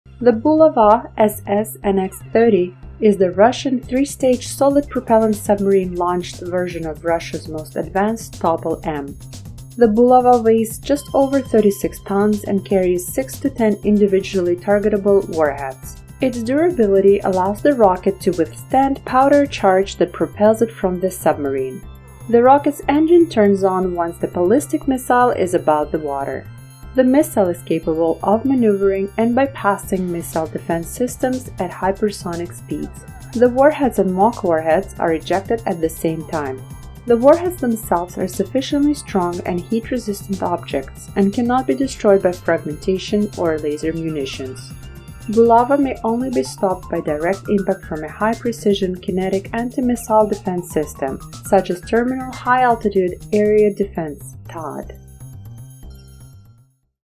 How Bulava SLBM works?
Bulava submarine-launched ballistic missile is the most expensive weapons project 16 missiles per vessel.